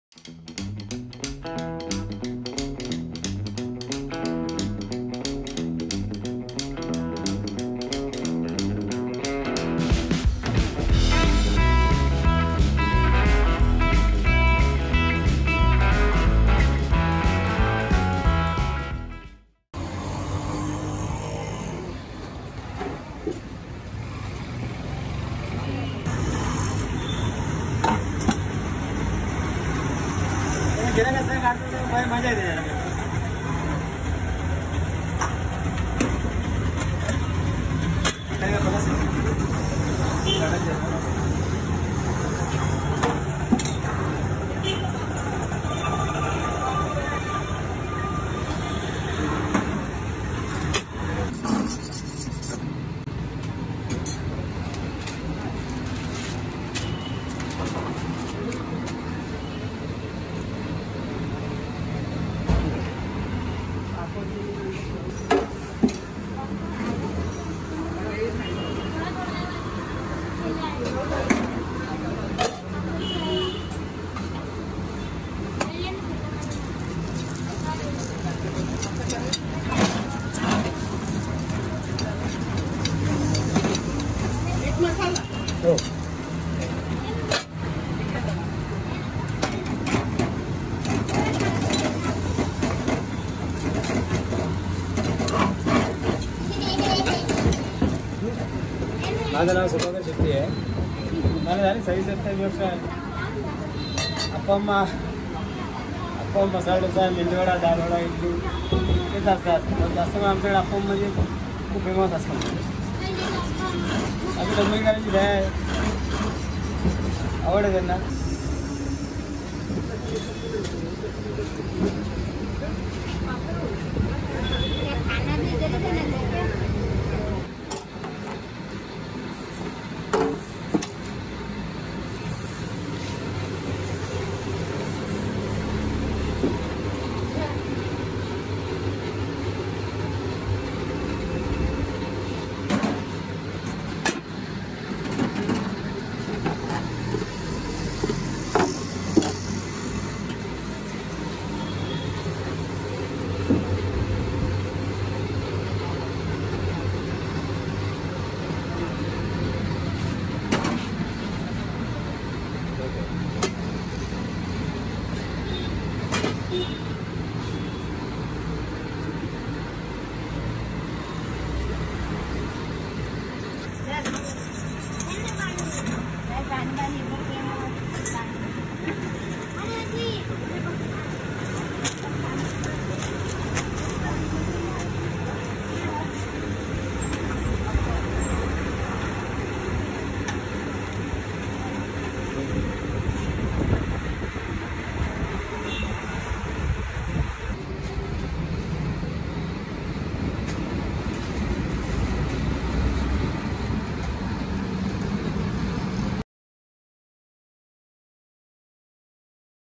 Masala Appam & Appam Recipe | Street Food,மசாலா அப்பம் செய்முறை | Samayam Tamil
Masala appam & Appam recipe in tamil
மசாலா அப்பம் & அப்பம் செய்வது எப்படி மும்பை தெருக்களில் விற்கப்படும் அப்பம் செய்முறை